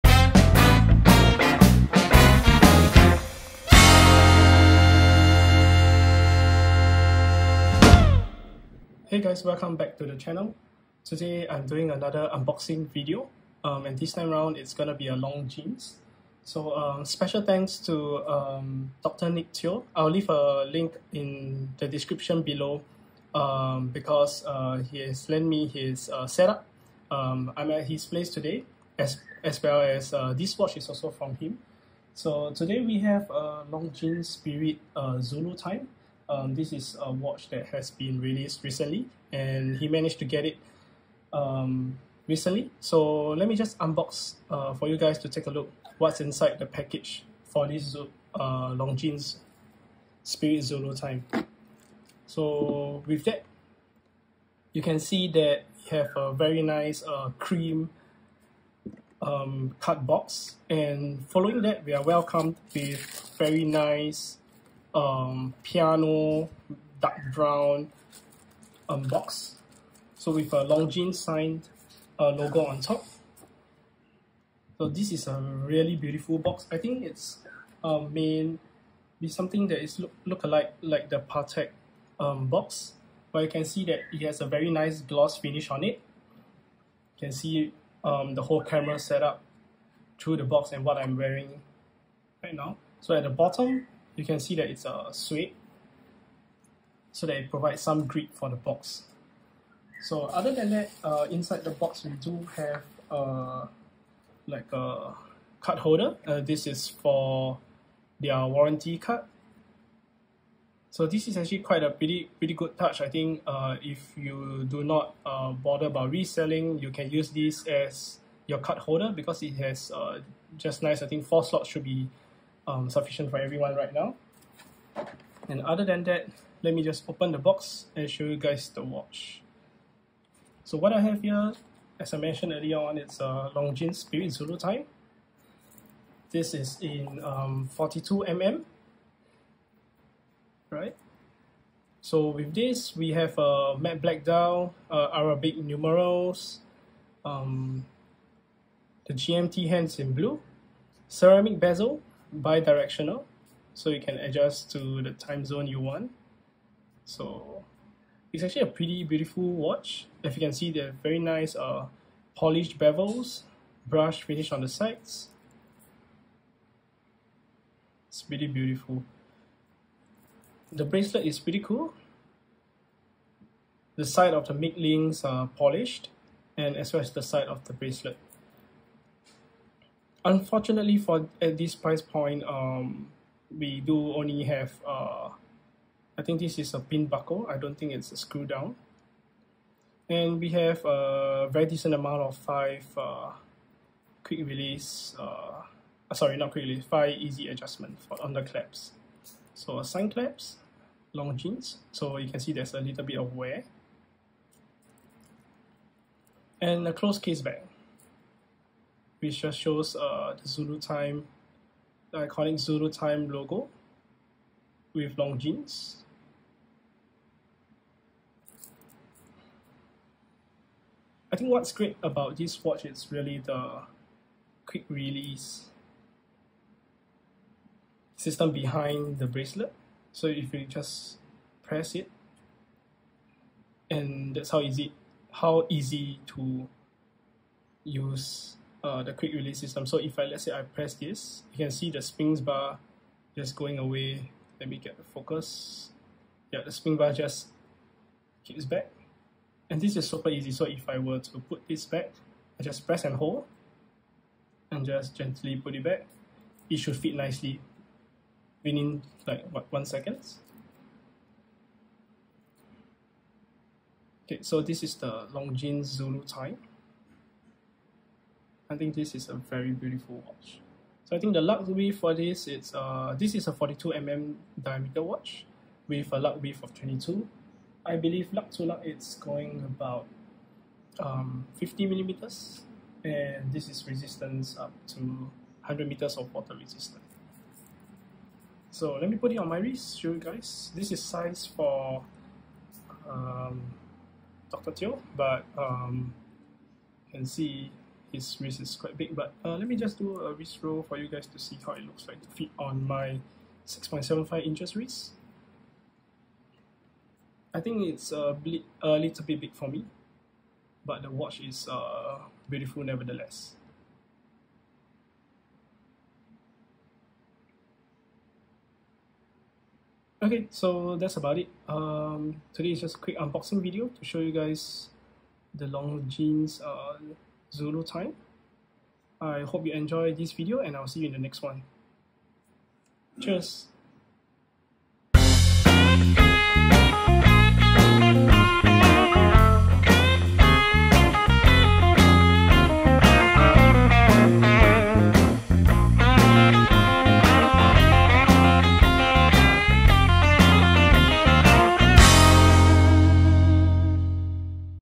[Longines] Spirit Zulu Time
Hey there! Welcome back to the channel.

Recently released Longines Zulu Time, a GMT watch that has been growing on me since I recorded this video. Had the chance to spend some time with it and realised how much of a watch it is with the price tag that you're paying. I told myself, please do not nwa. Hope you will enjoy this unboxing video as well as the watch shown. Cheers!